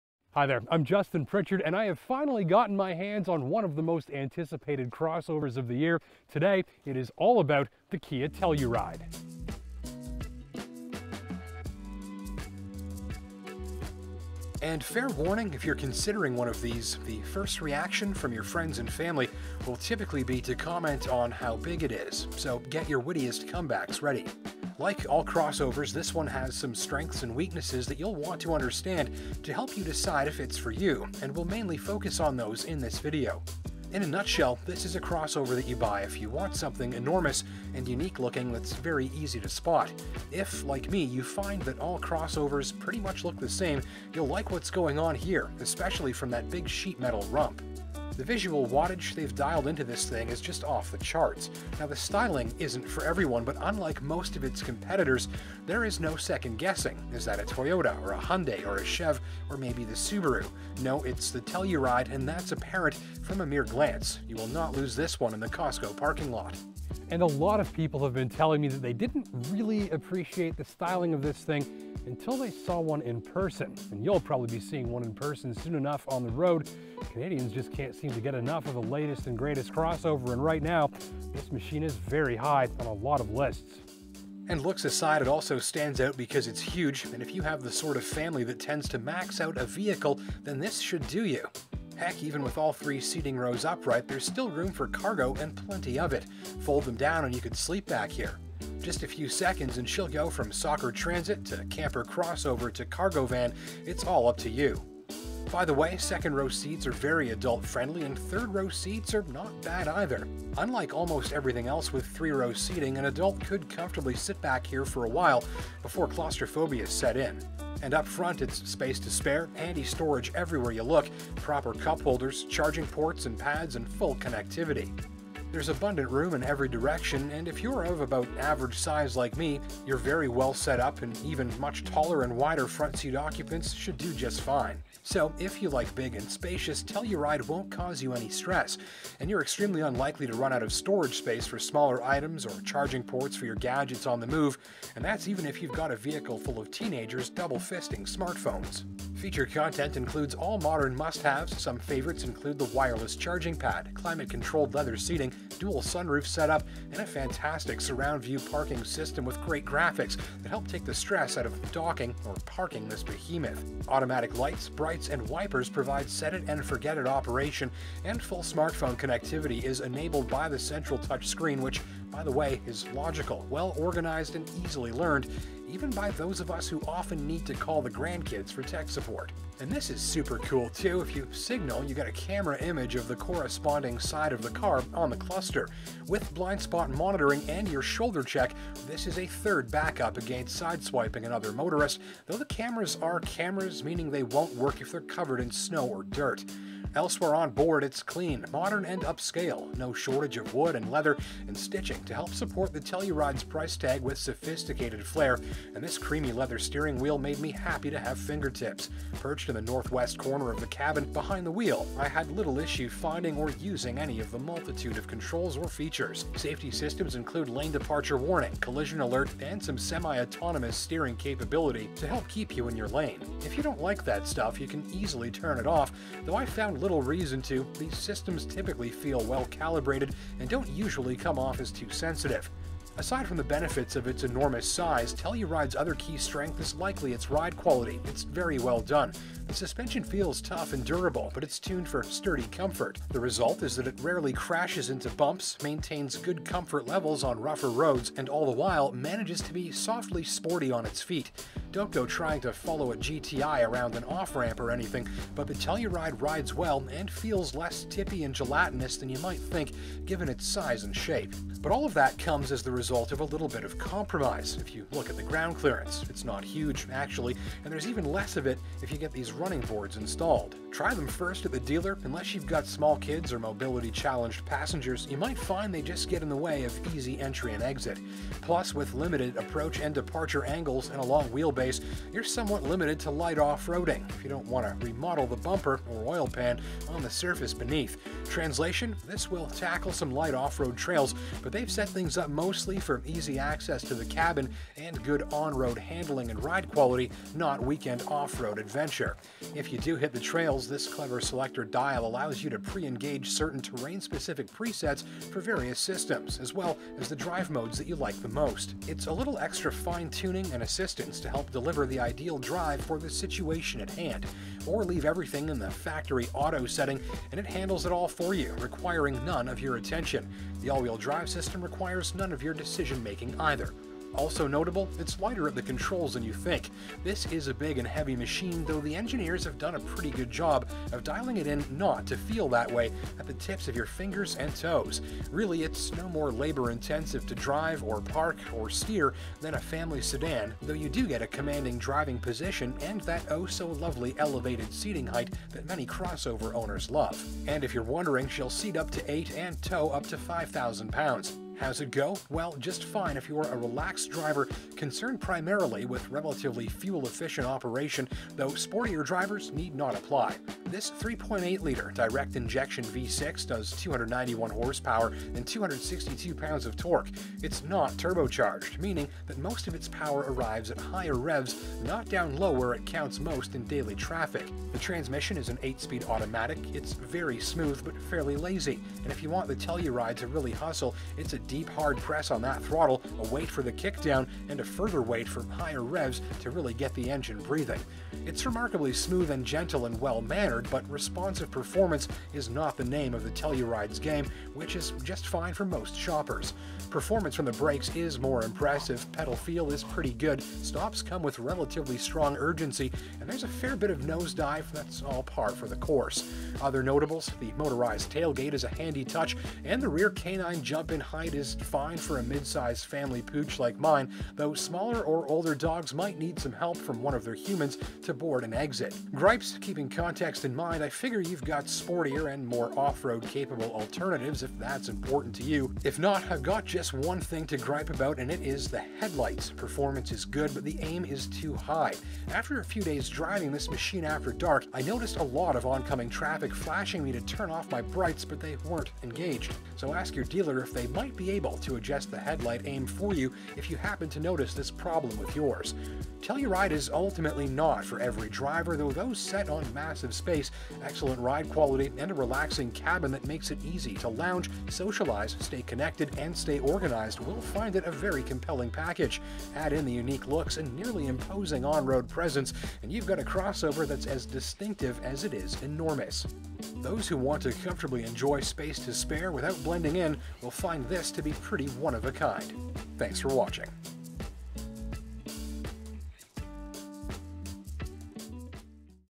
2019 Kia Telluride Review (Northern Ontario)
A full review of the 2019 Kia Niro SX from beautiful Northern Ontario. Includes a full review, driving impressions, and demonstration of the 2019 Kia Telluride SX AWD, with Canadian specs and pricing.